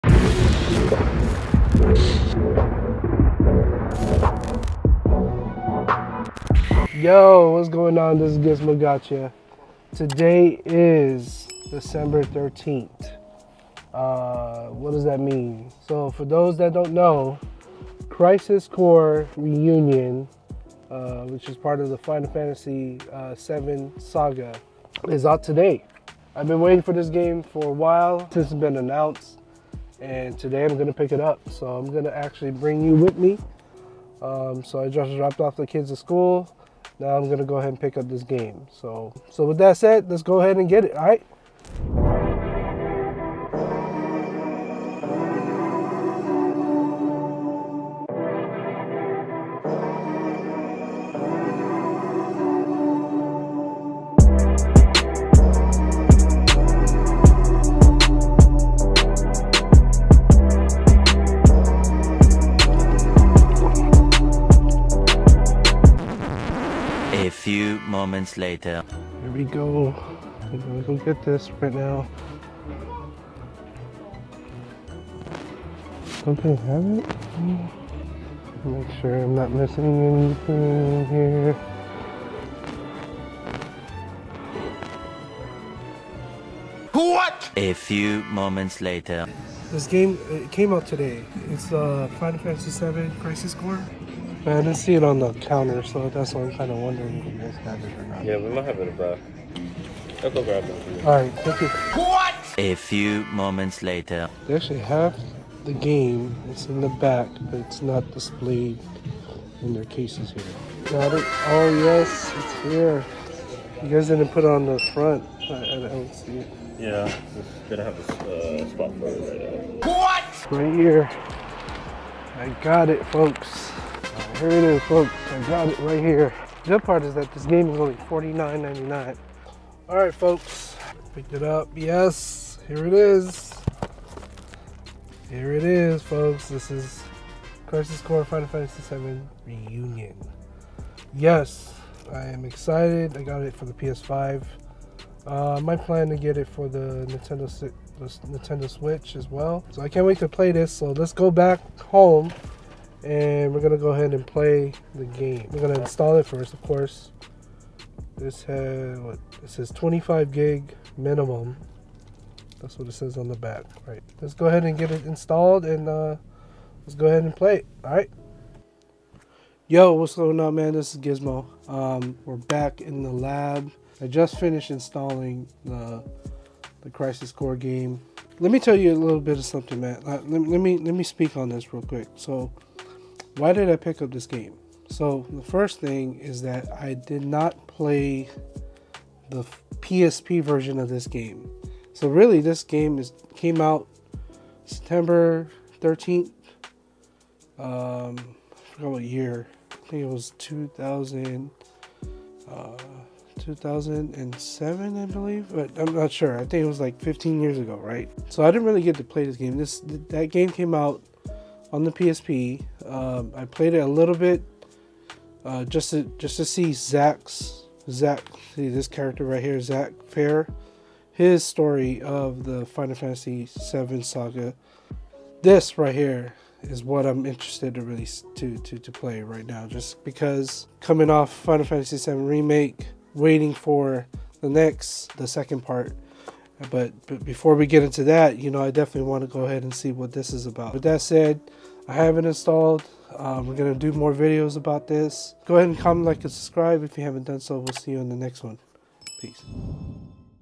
VLOG 1 - Picking Up That FFVII Crisis Core Reunion
We take you on a our pickups to seek Final Fantasy Crisis Core Reunion in our first ever vlog style video. Look out for gameplay videos coming soon.